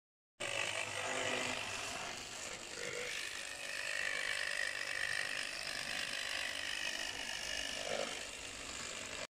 JŽ 665
N scale model of Yugoslav Railways locomotive.
Made by Milorad Jovanović, Belgrade, Serbia, in 1982.